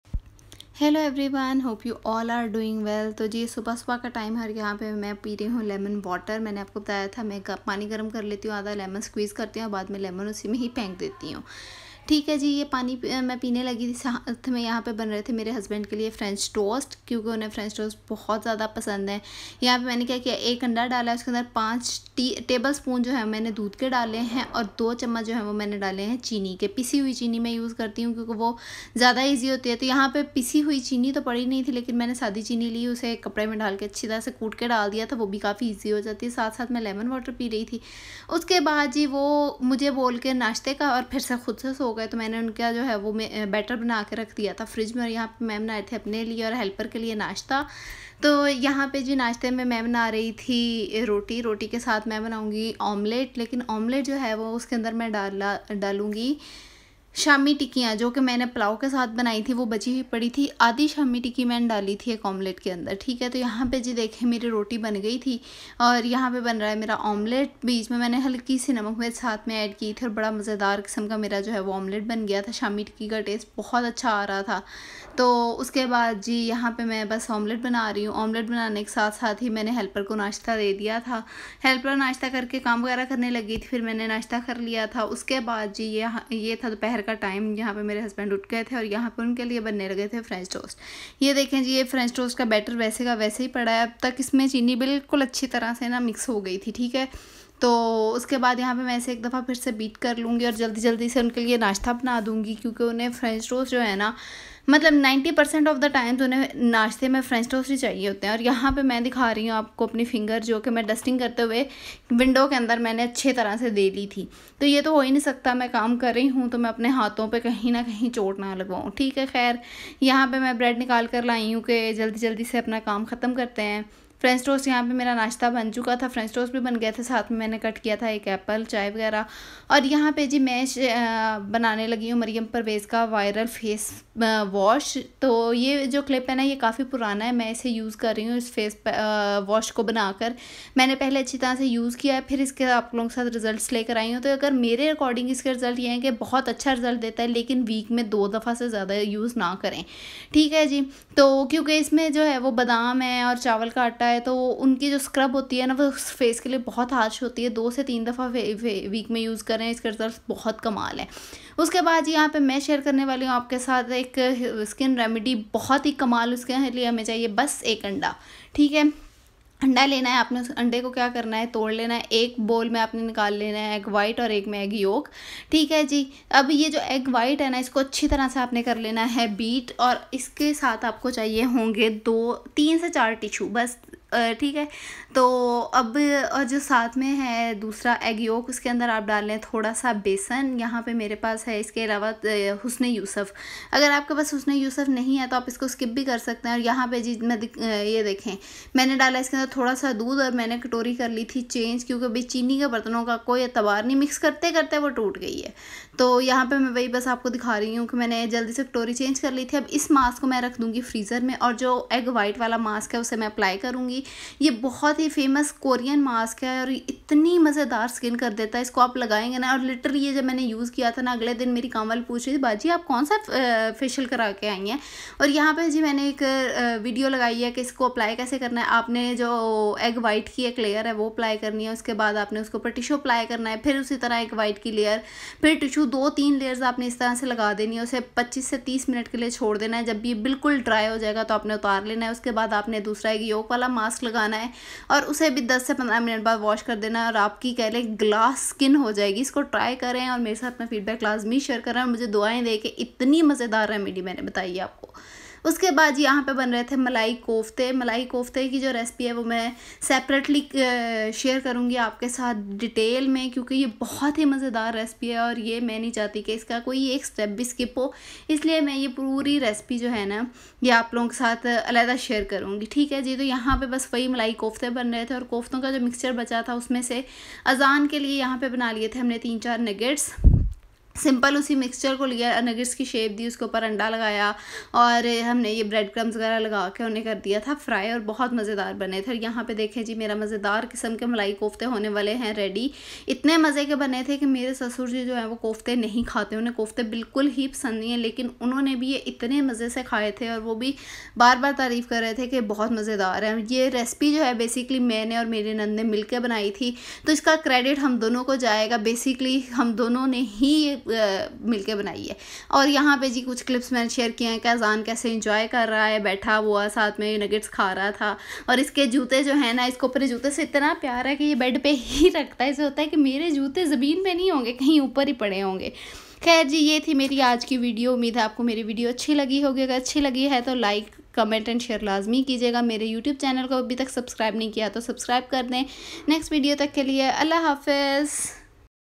French Toast Recipe | Review Merium Pervaiz facewash |Malai kofta | koren skincare for glass skin
Perfect French Toast Recipe | Review Merium Pervaiz viral facewash | Mazydar Malai kofta | koren skincare for glass skin

Merium Pervaiz Facewash:

Dry milk — 2tbsp
Gram flour (beesan) — 3tbsp
Badaam powder — 1tbsp
Rice flour — half tbsp